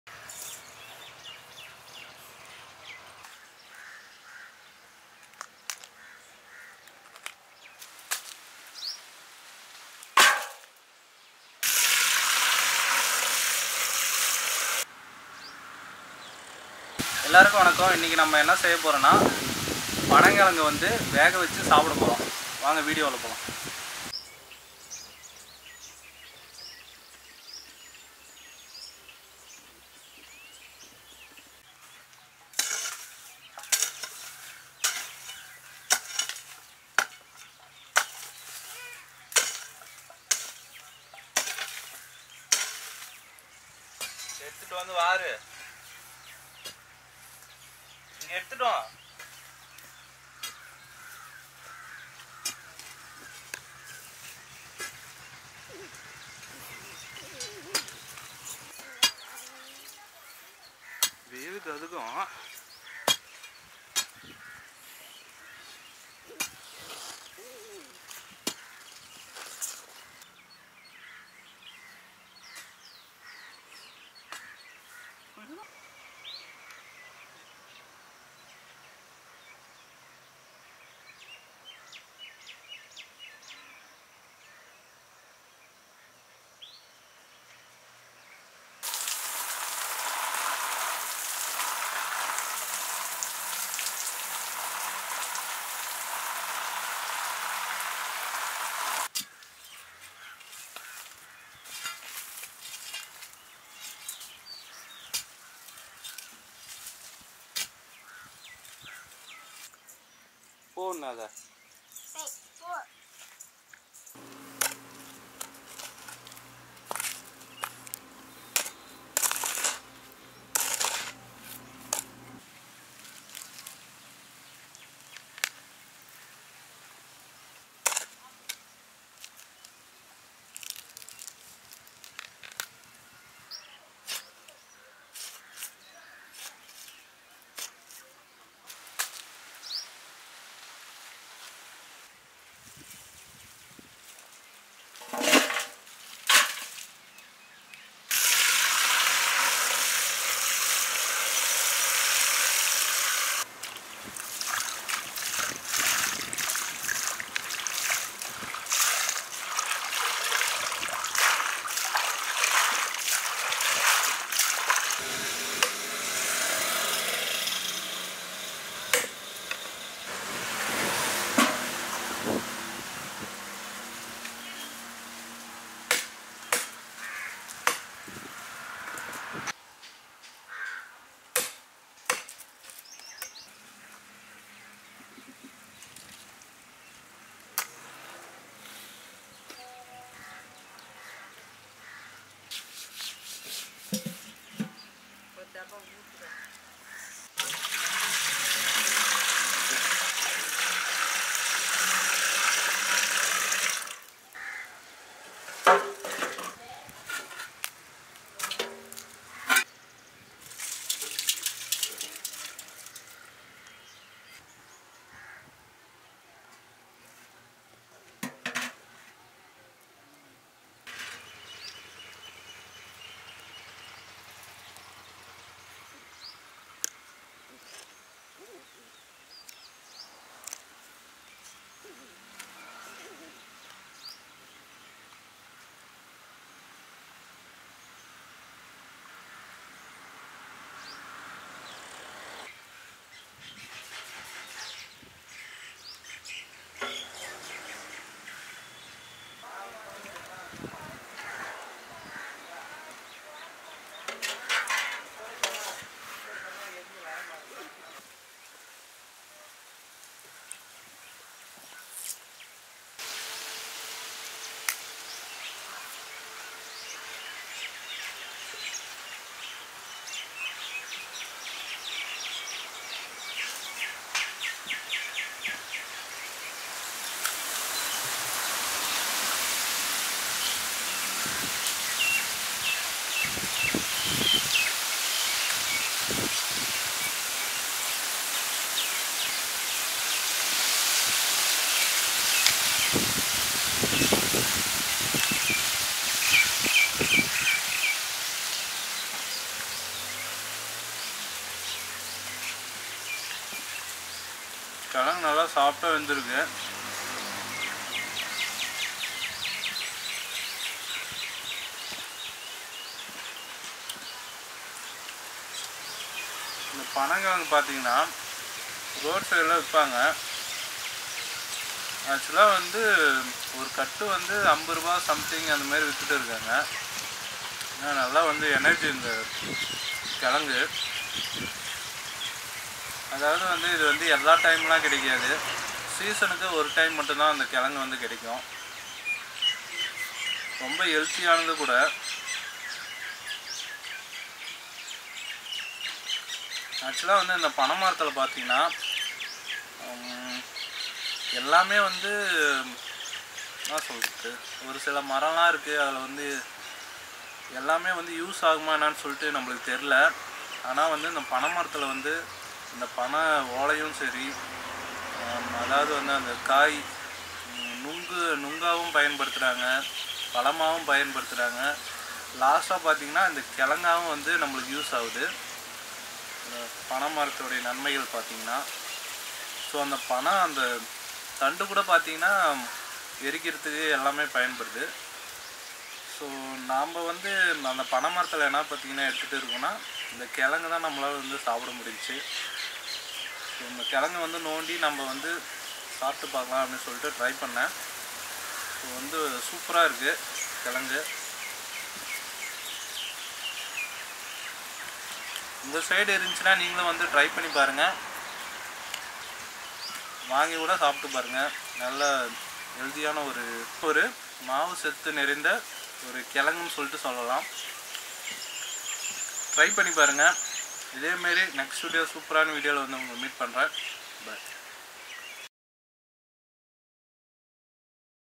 🔥Panang Kilangu ( Palmyra sprout ) || Cook With Tamil Food Vlog
The Palmyra Sprout is obtained from the Palmyra palm or toddy palm or Borassus Flabellifer. The Palmyra palm tree is similar to the Coconut tree in look wise but while comparing it is shorter in height and have a fan-shaped leave along with pointed Palmyra Sprout is derived from the Palmyra palm, also known as the toddy palm or Borassus Flabellifer. The Palmyra palm tree is similar to the Coconut tree in, however, it is shorter in height and has fan-shaped leaves with pointy tips.